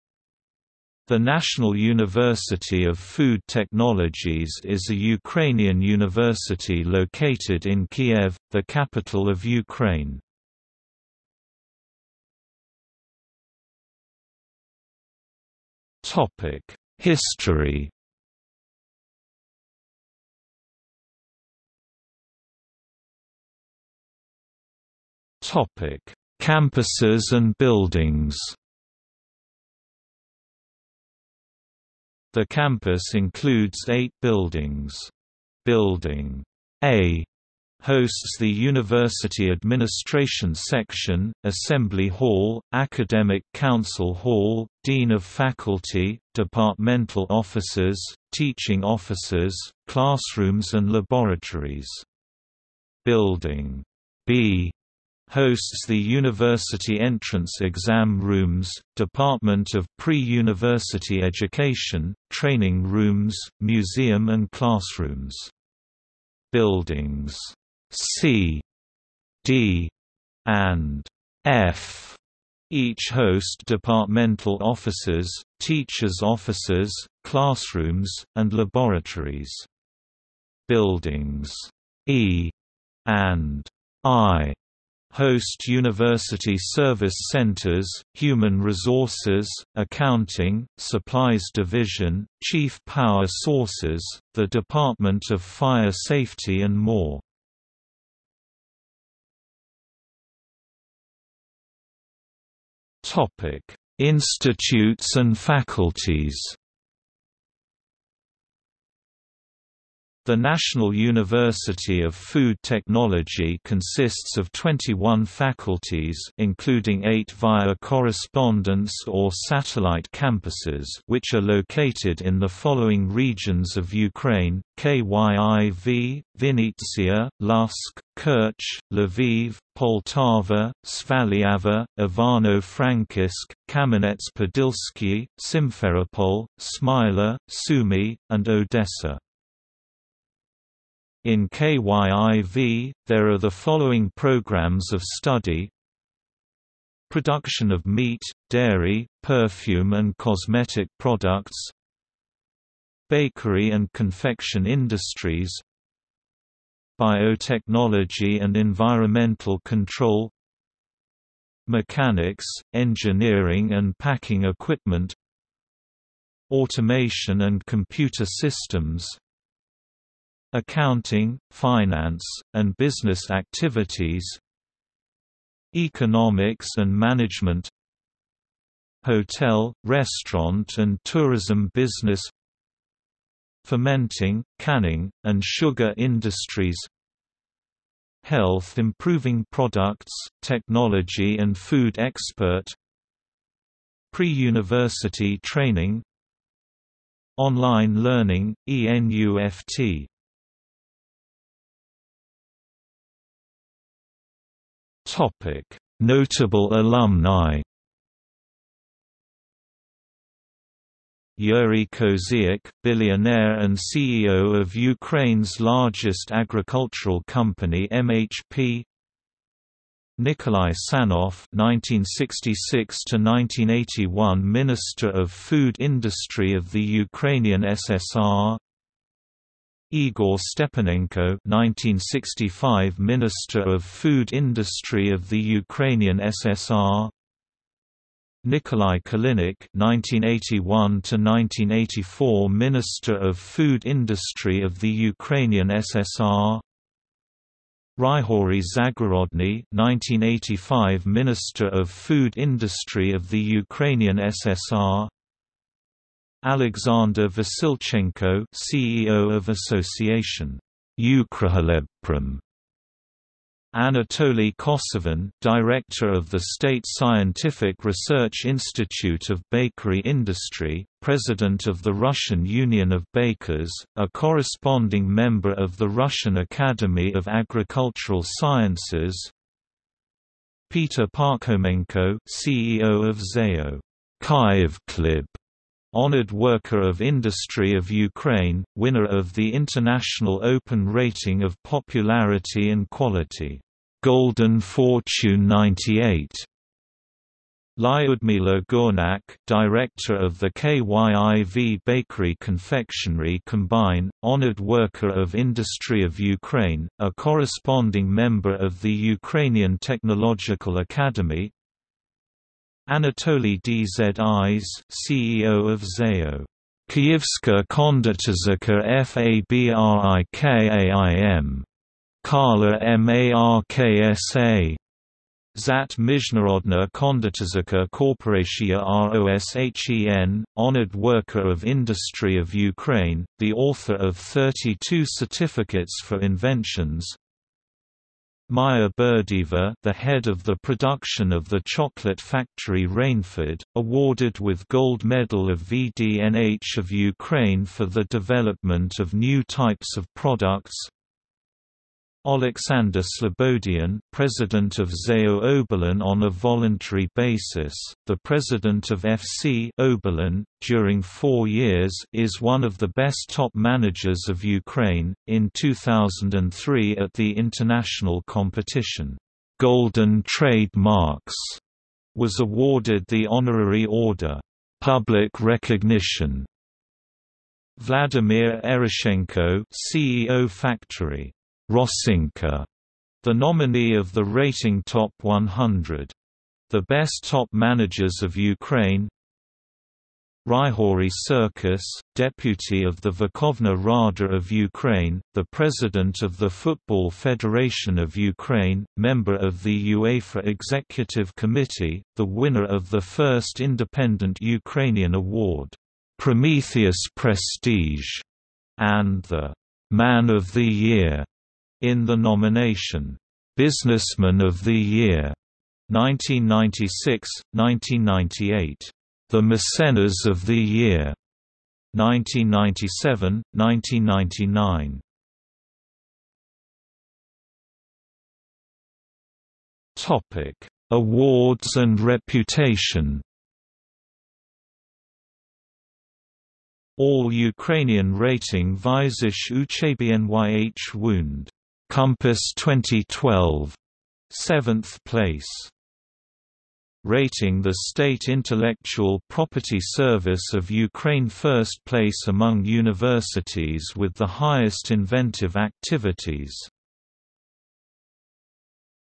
National University of Food Technologies | Wikipedia audio article
00:00:12 1 History
00:00:21 2 Campuses and Buildings
00:01:39 3 Institutes and Faculties
00:03:21 4 Notable alumni
00:07:06 5 Awards and Reputation

- Socrates

SUMMARY
The National University of Food Technologies is a Ukrainian university located in Kiev, the capital of Ukraine.